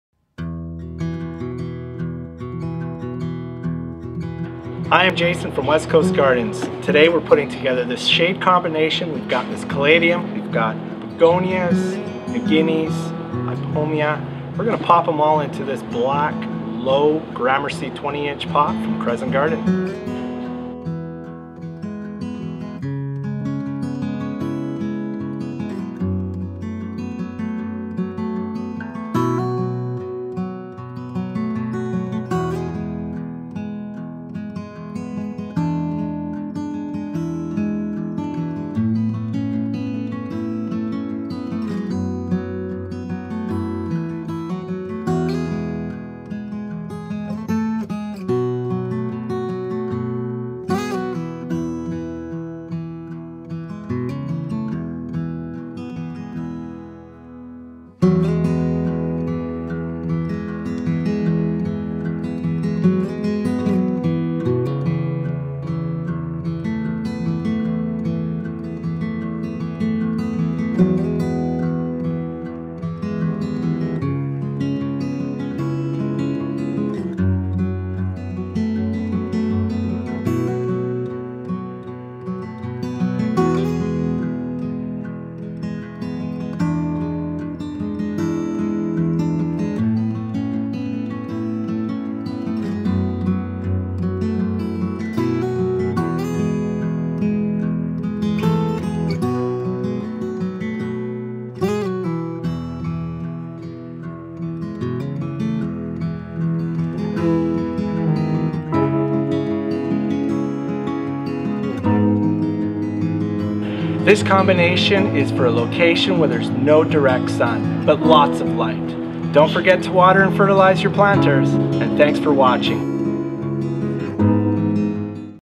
Easy To Create Colourful Shade Planter for Your Patio || West Coast Gardens
Light up your life with this gorgeous planter designed to thrive in the shade. It's absolutely bursting with colourful coleus, begonias and more, including an eye-catching caladium to top it all off. Looking at the amazing selection of plants, it's hard to believe they can all fit in the 20" pot, but keep watching to see Jason work his magic.

For those keeping score, that's 4 different varieties of coleus, 3 types of begonias, 2 sweet potato vines and 8 more stunning plants. Feel free to improvise your own choices as well, but remember to check the plant info to make sure they will be happy in your shady patio oasis. 🌸

Here's our list of colourful ingredients for this stunning 20" shade planter.

Caladium "Miss Muffet"
Cyperus "Wild Spike"
Impatiens "Harmony Radiance"
Selaginella
Millet "Flashlights"
Oxalis
Fuchsia "Autumnal"
Fiber Optic Grass
Coleus
"French Quarter"
"Electric Lime"
"Lava Rose"
"Burgundy Wedding Train"
Begonias
"Miss Montreal"
"Unstoppable Upright Salmon"
"Unbelievable Lucky Strike"
Sweet Potato Vines
"Marguerite"
"Black"

♥️ RESOURCES || LINKS ♥️